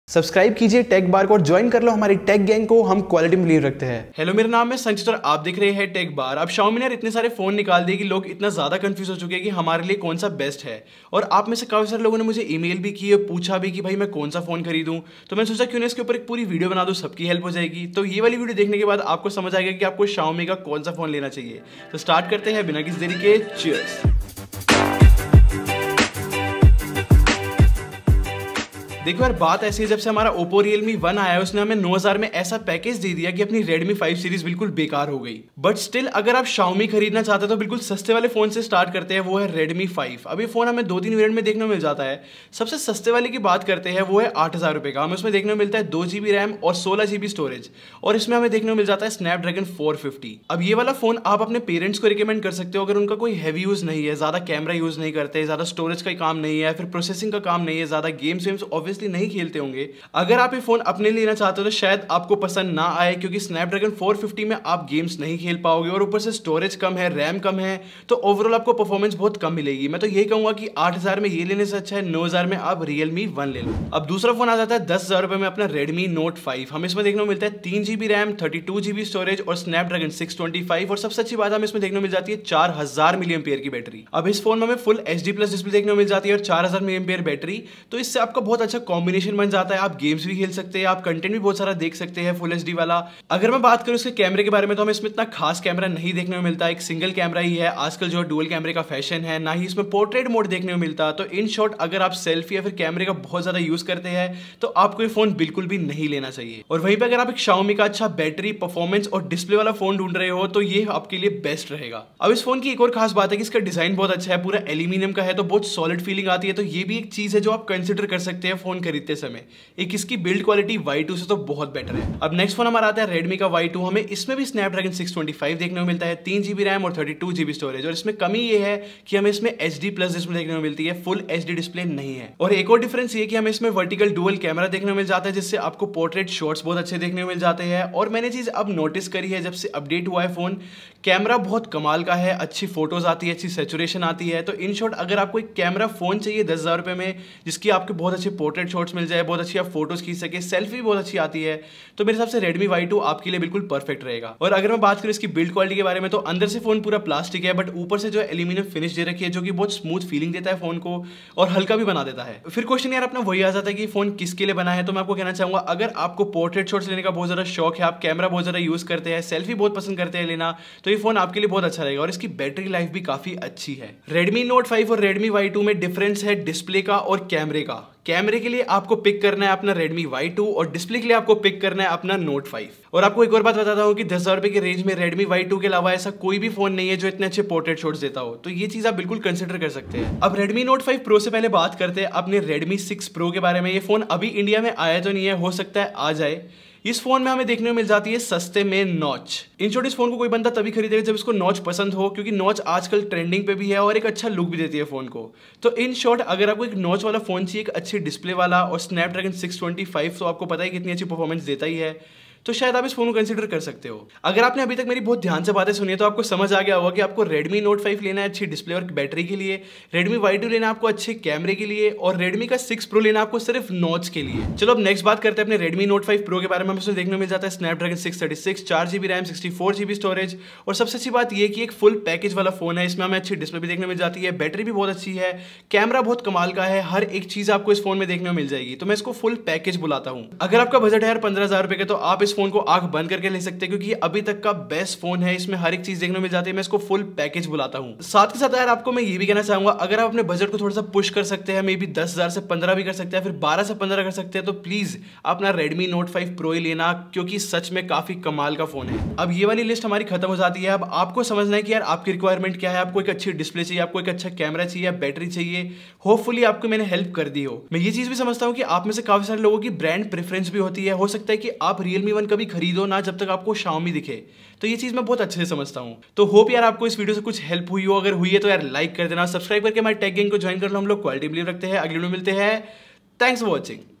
Xiaomi Ka Kaunsa Phone Kharidna Chahiye? - Note 5, Redmi Y2, Redmi 6 Pro, Note 5 Pro
In this video, I am expressing my thoughts on xiaomi smartphones. If you are a xiaomi fan then it must be a tough time for you because these days xiaomi is launching a lot of smartphones and people are getting confused which variant is the best. Well in this video I am talking about redmi 6 pro, redmi note 5 pro, xiaomi redmi note 5 and redmi y2. So cheers!